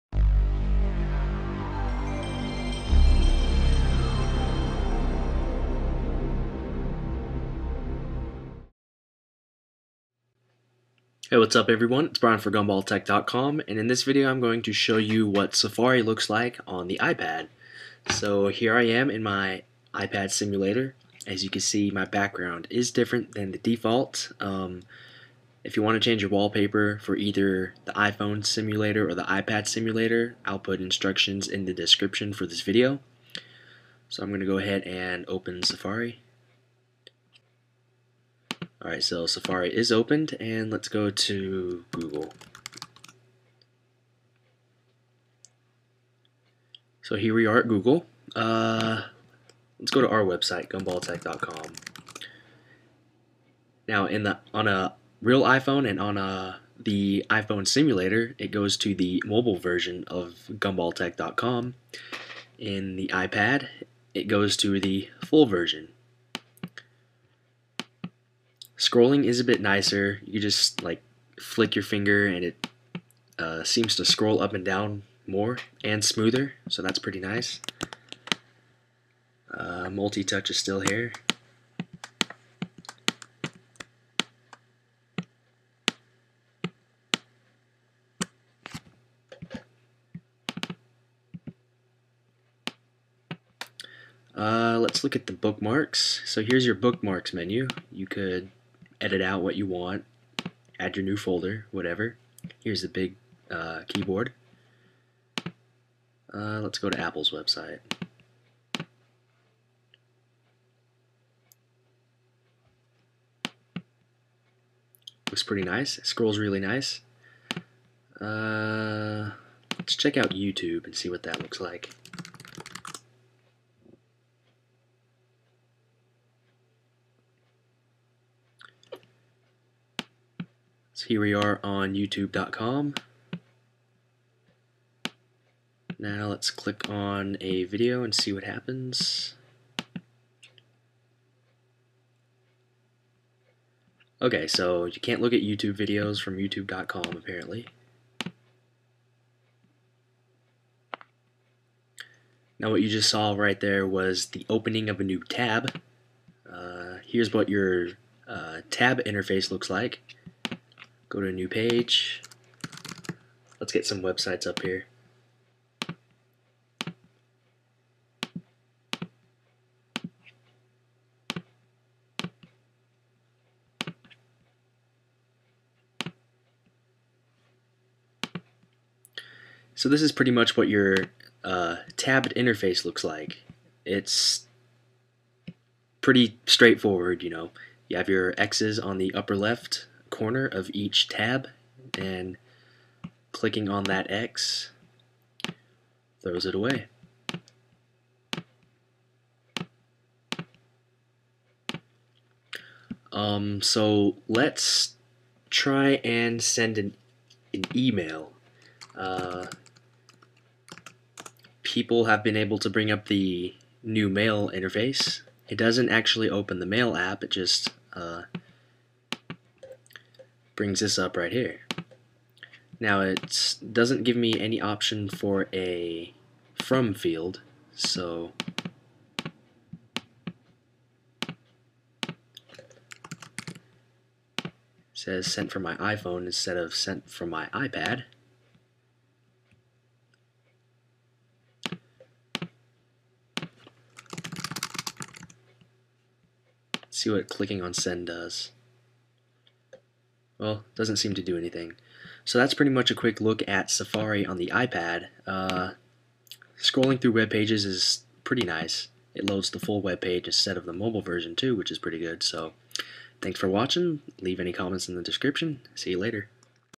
Safari on the iPad
Just a quick video showing Safari on the iPad using the iPad Simulator.